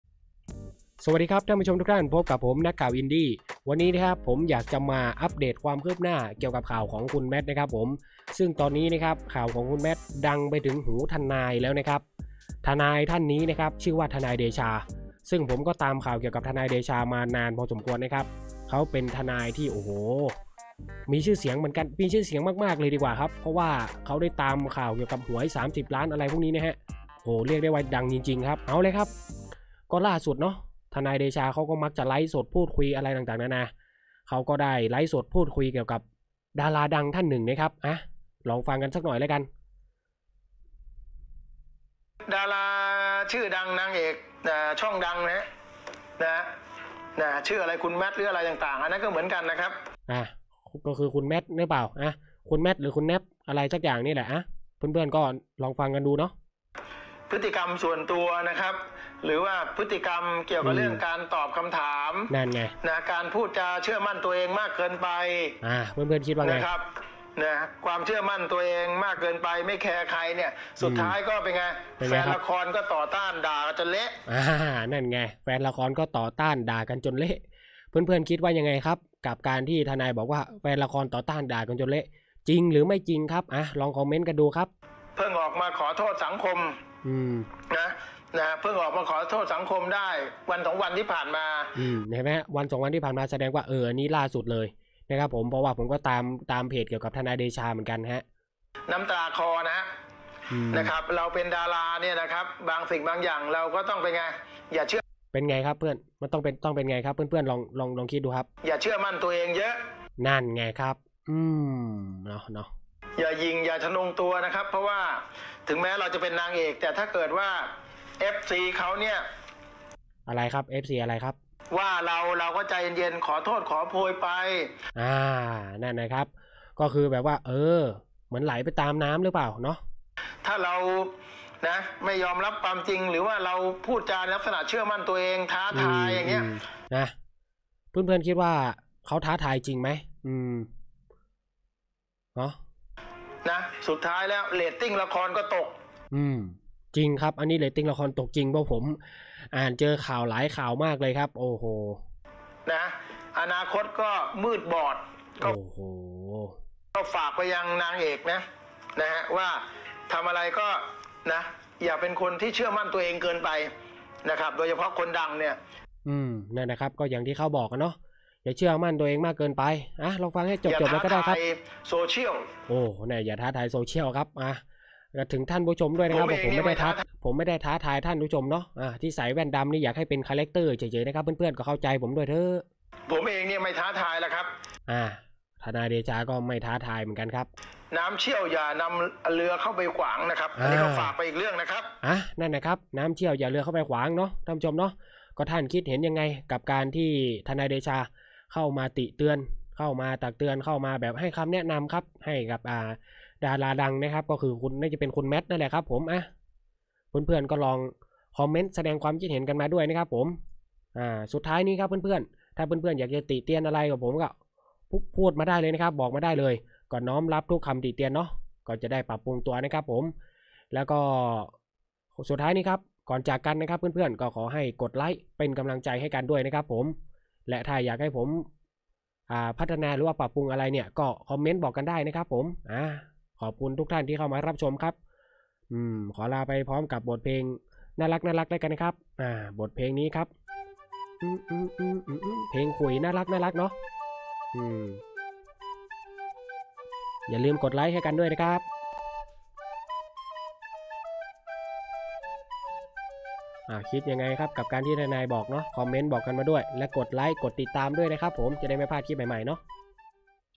ถือว่าสอน! ทนาย เดชา ฝากถึง แมท ชี้ "น้ำเชี่ยว อย่าเอาเรือไปขว้าง"
ชื่อข่าว : ทนายดังฝากถึงแมท! ทนาย เดชา ชี้ น้ำเชี่ยว อย่าเอาเรือไปขว้าง
สรุปเนื้อหาข่าว : ทนายดังฝากถึงแมท! ทนาย เดชา ชี้ น้ำเชี่ยว อย่าเอาเรือไปขว้าง
ขอขอบคุณข้อมูลดีๆจาก : ทนายเดชา

ทนายเดชาแนะนำแมทภีรนีย์
ทนาย เดชา
แมท ภีรนีย์
แมท สงกรานต์
สงกรานต์ เตชะณรงค์